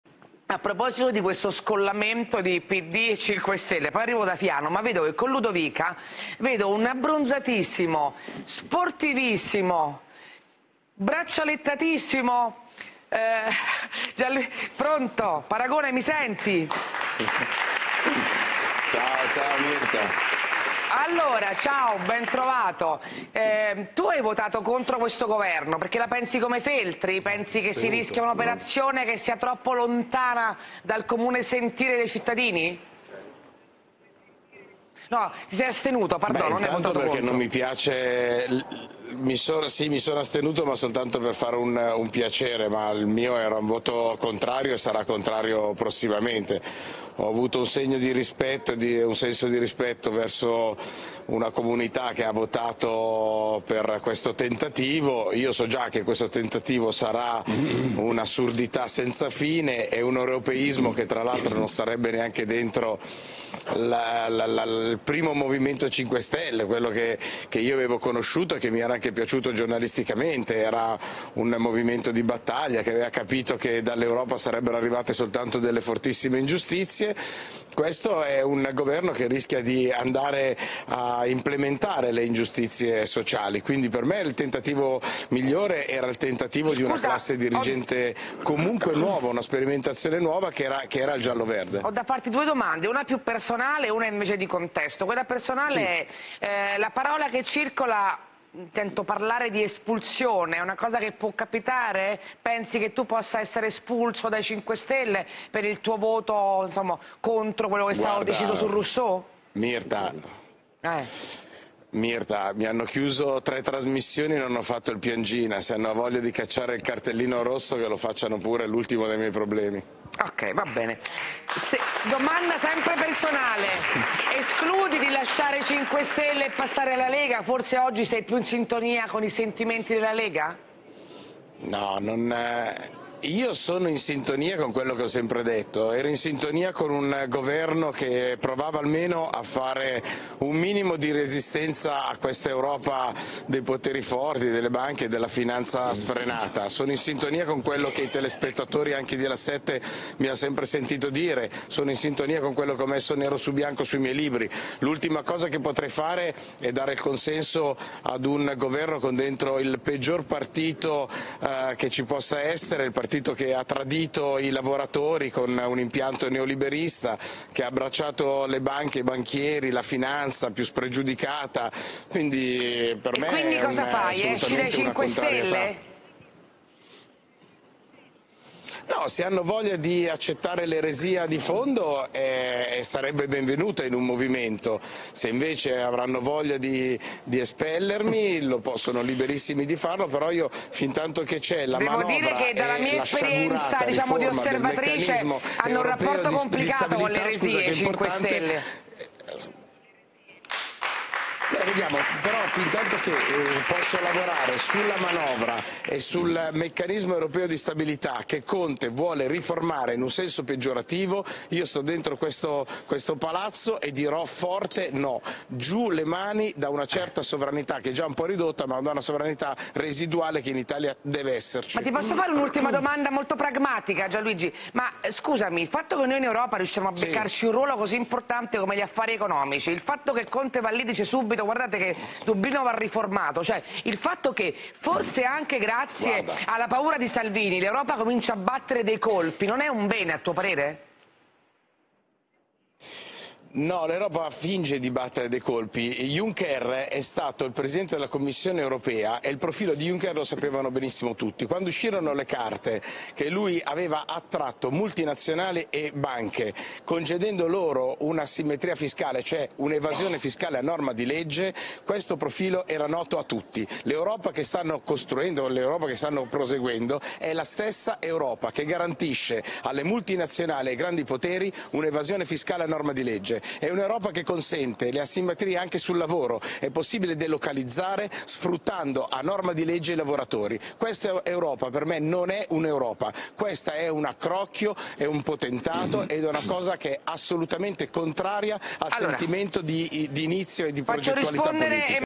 Gianluigi Paragone, M5S: "Se hanno voglia di cacciare il cartellino rosso che lo facciano pure"
Gianluigi Paragone (M5S): "Mi sono astenuto ma soltanto per fare un piacere e per rispetto nei confronti della comunità che ha votato a favore di questo tentativo, il mio voto è contrario".